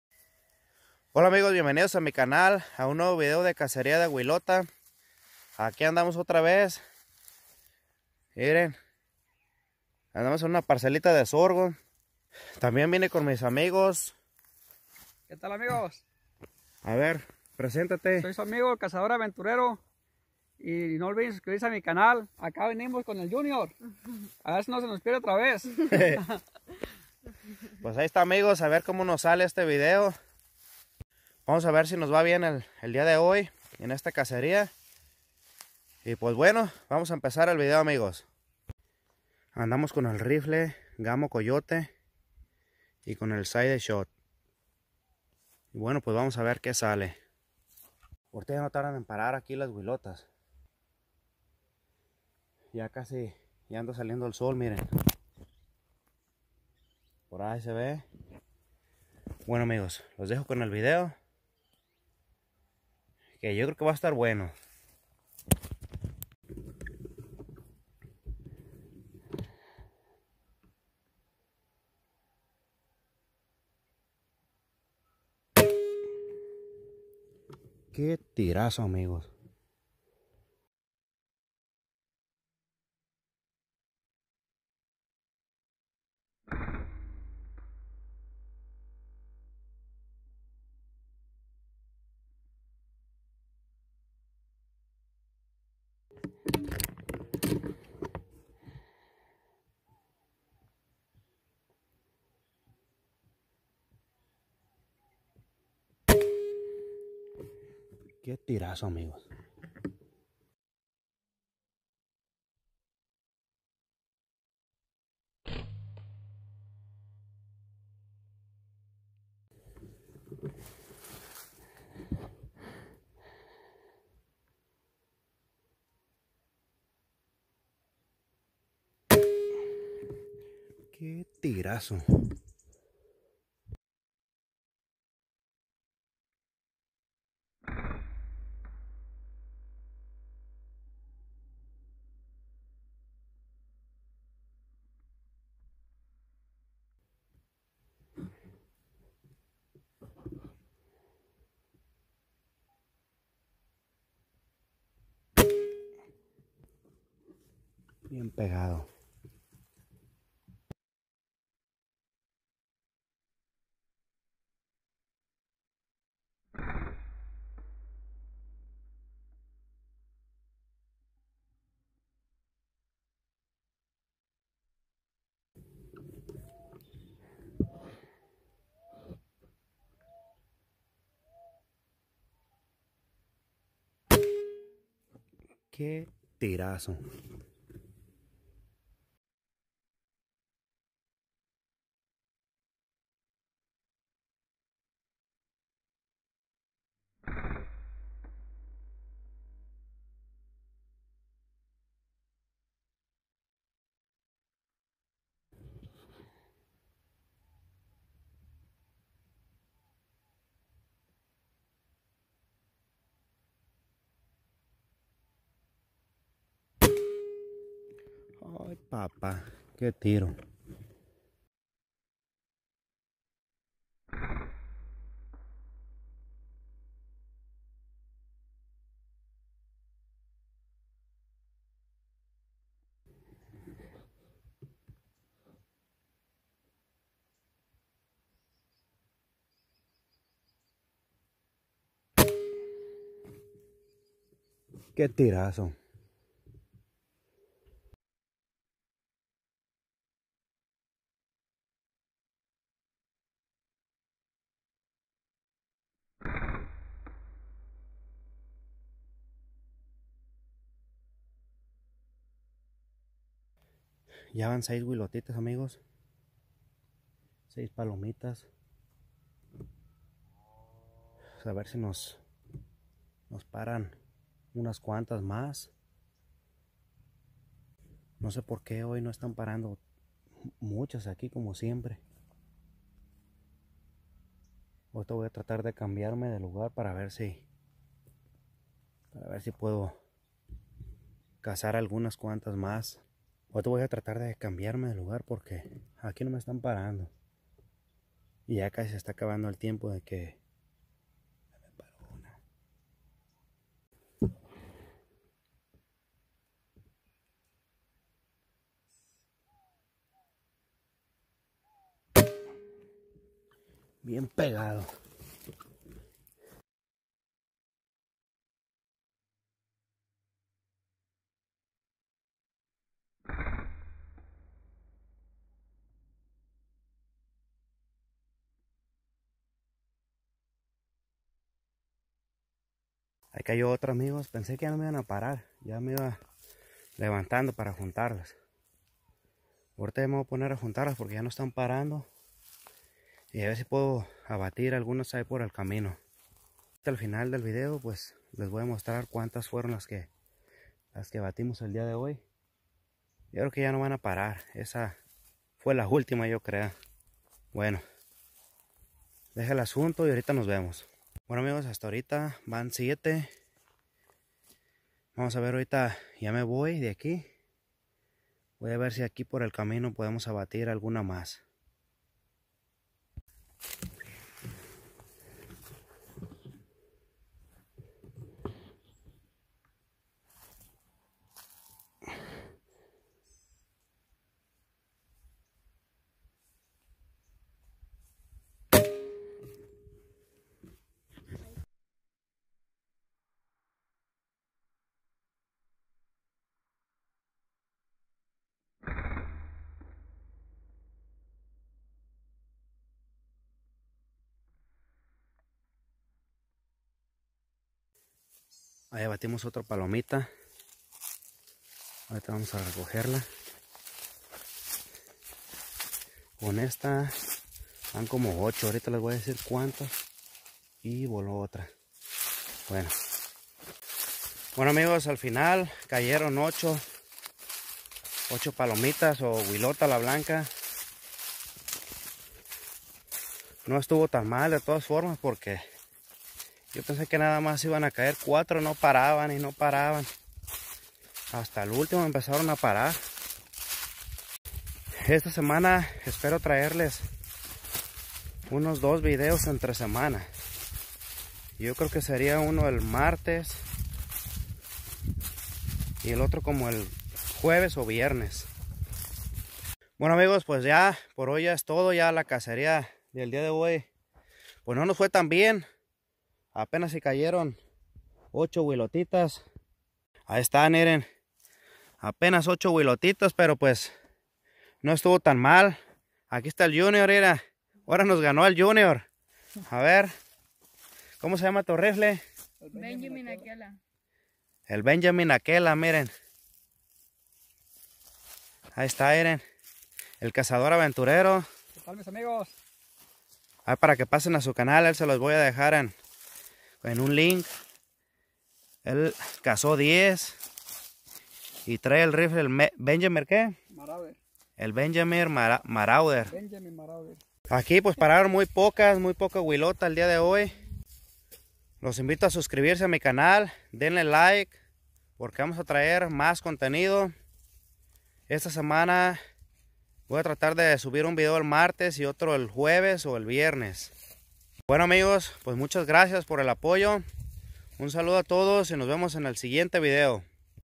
Caza De Palomas Con Rifle De Aire PCP
Hola amigos aquí les dejo un nuevo video de caceria de paloma Ala Blanca con rifle de aire pcp una nueva cacería espero les guste.